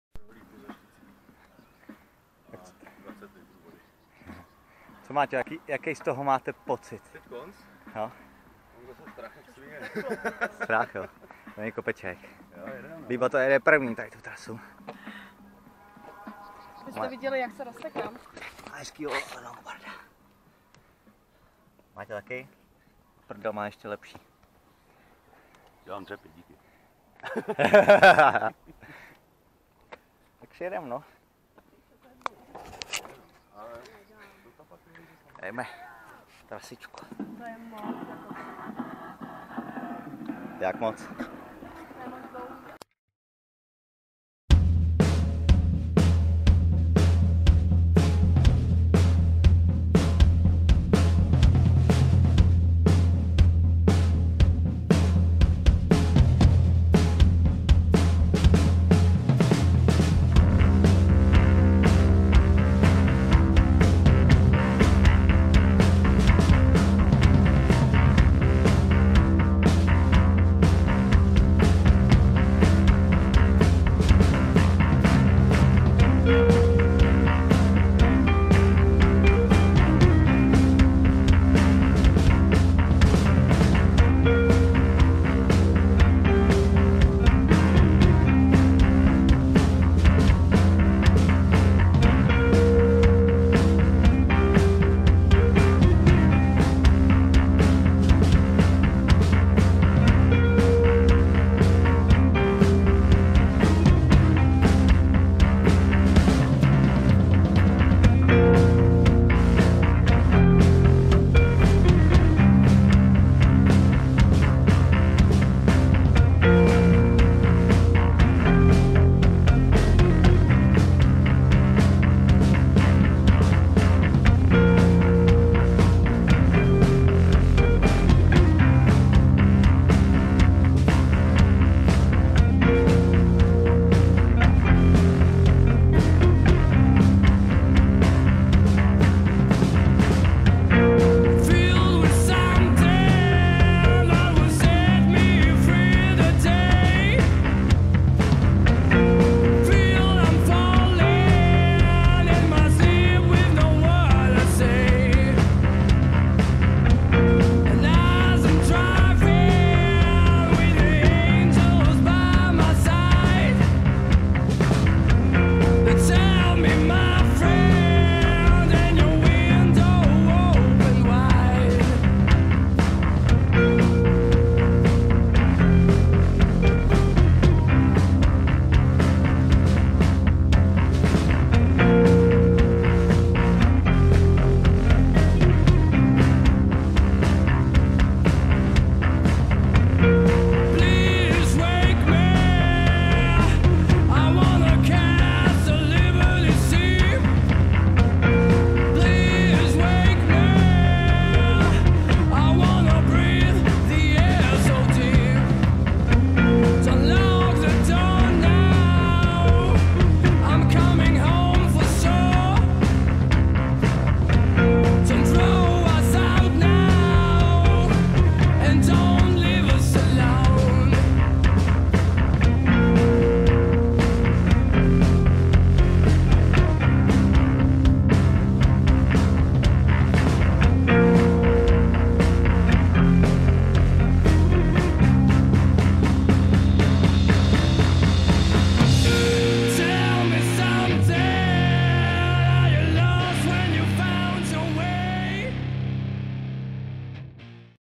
El longbordo pondělos
První pokusy na longboardu. Dva sjezdy. Je to jen na rychlo sestříhaný. Spíše random videjko.